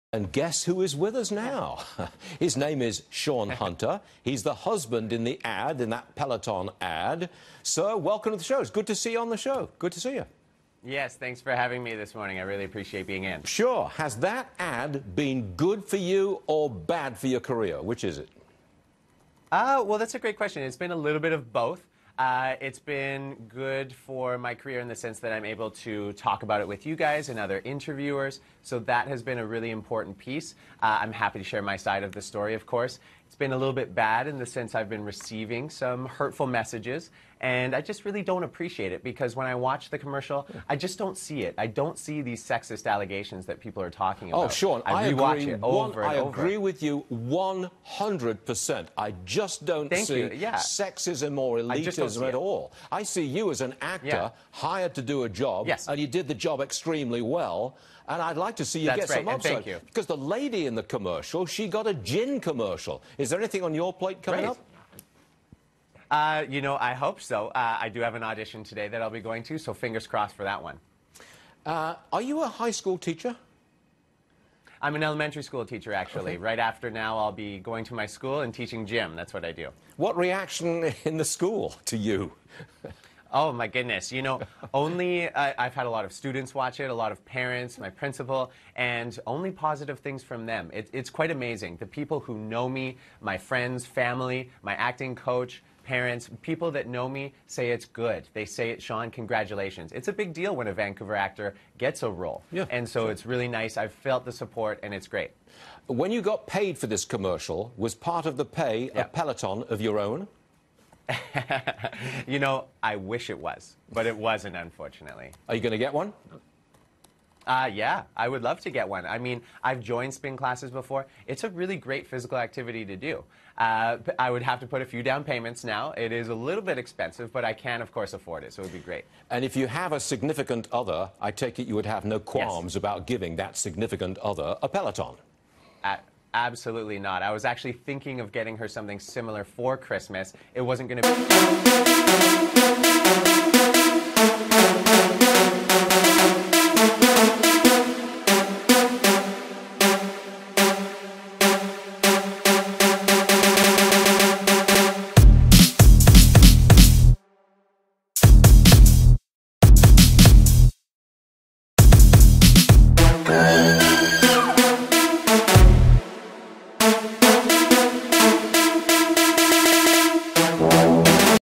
Peloton husband received ‘hurtful messages’ after new ad aired HD 1080
Husband in Peloton ad Sean Hunter discusses how the now controversial commercial is impacting his career.